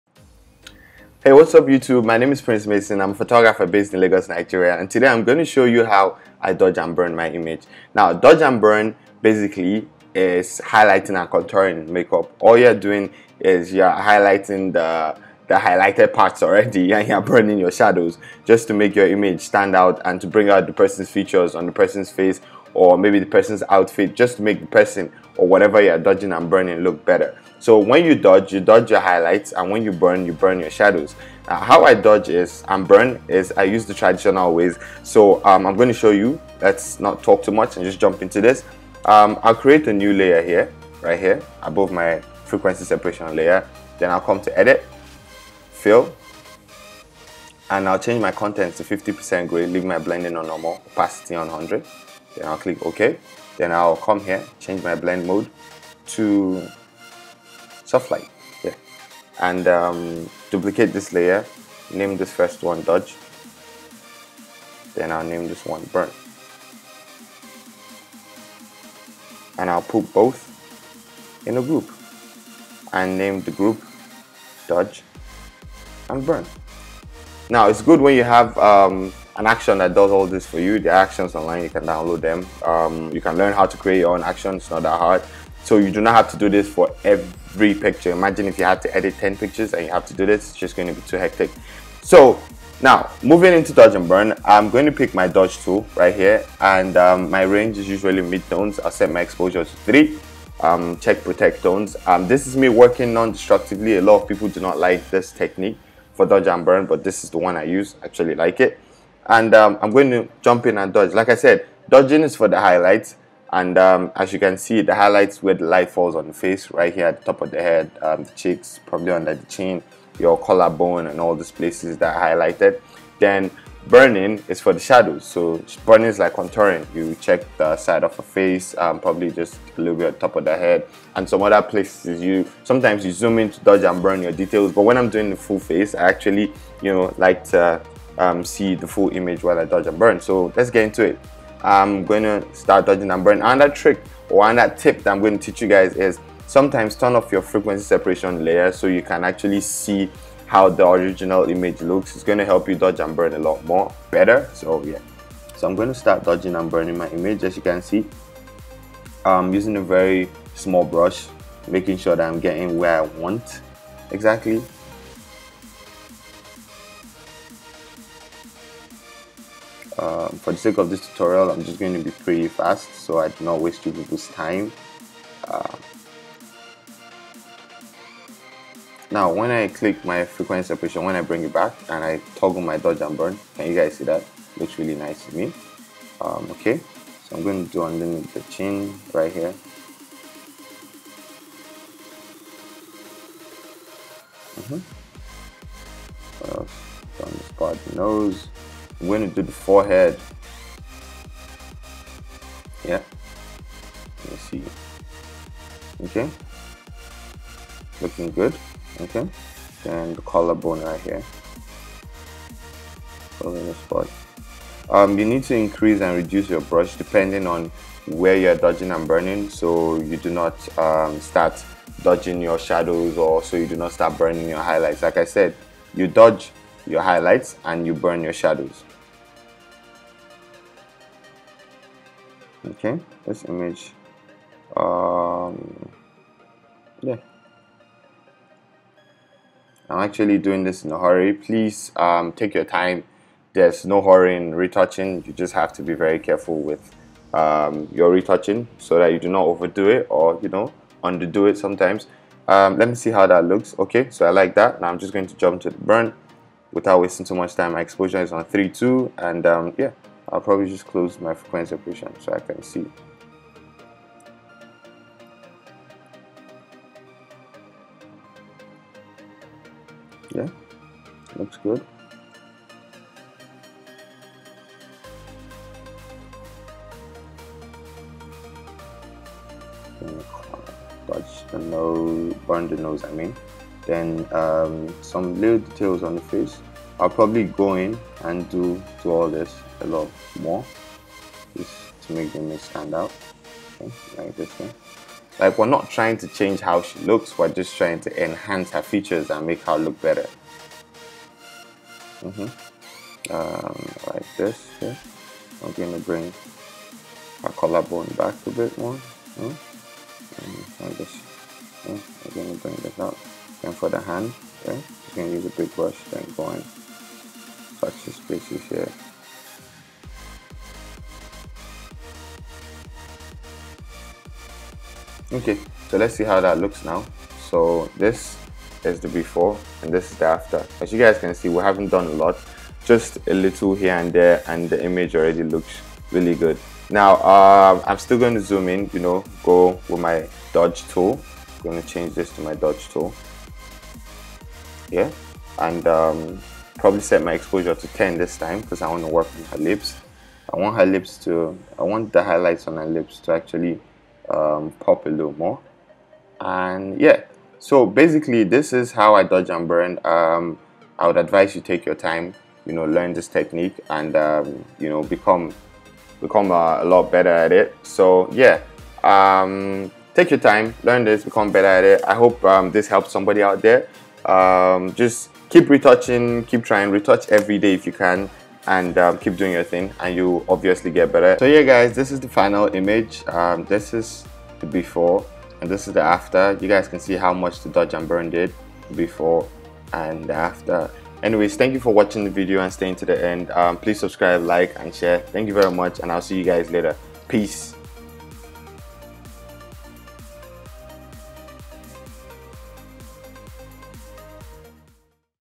Photoshop CC Tutorial: How to Dodge and Burn In 5 Minutes
In this Photoshop CC tutorial i'll show you how i Dodge and Burn my images in 5 minutes and get amazing Results with it.